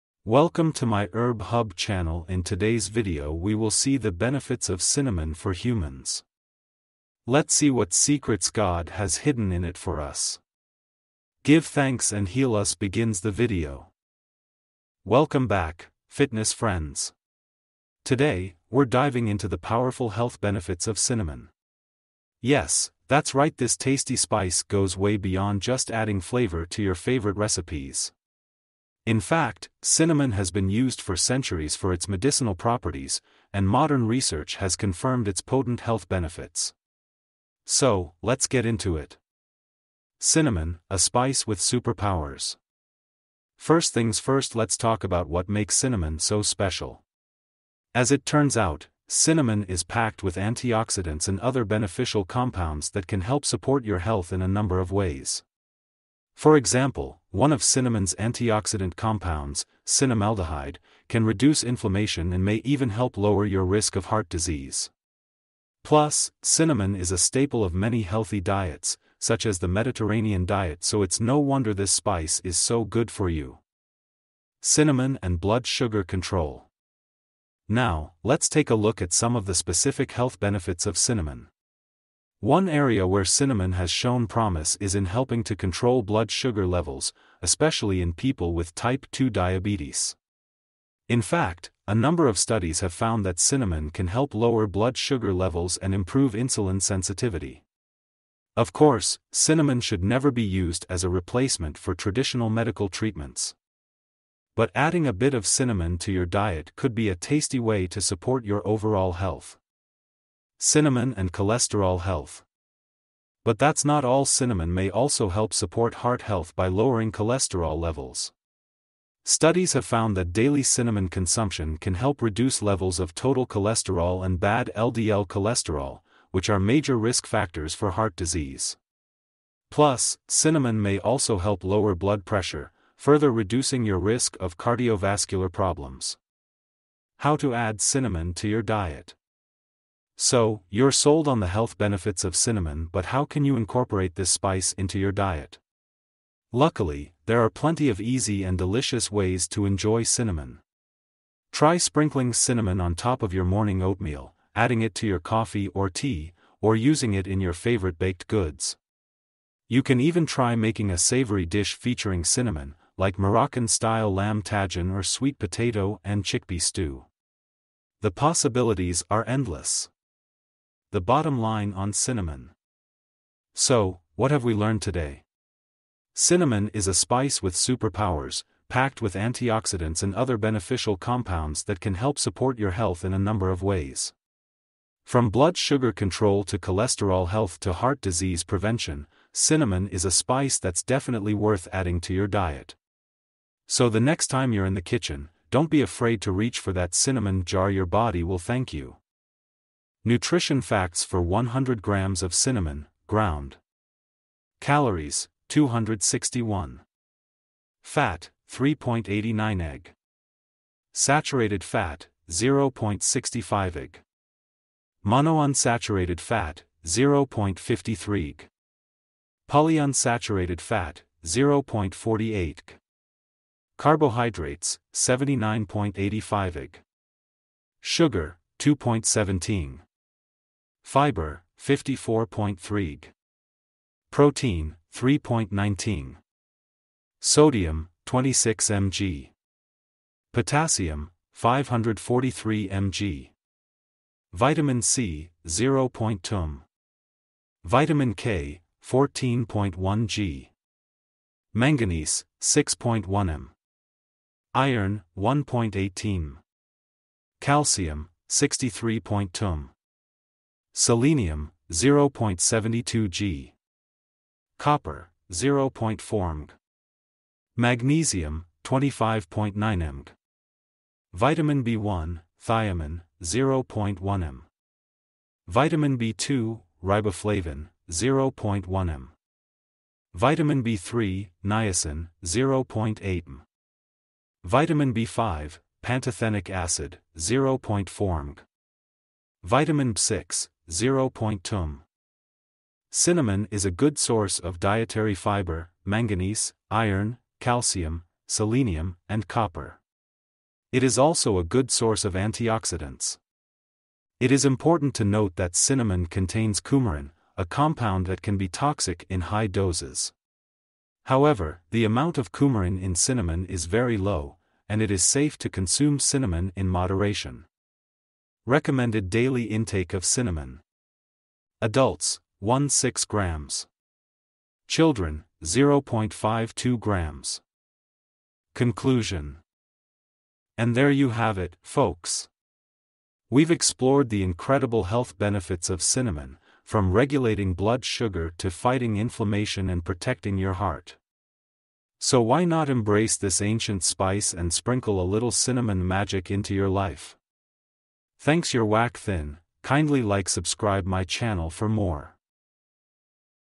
Cinnamon Nature's Secret Spice for Healthy Life | cinnamon tea recipe | cinnamon benefit | herb hub
In this video, we unveil the potent health benefits of cinnamon - a spice cherished for centuries. From balancing blood sugar levels and fighting inflammation to protecting your heart, cinnamon is truly a superfood. Join us on this cinnamon-infused journey to discover how this aromatic spice can enhance your well-being.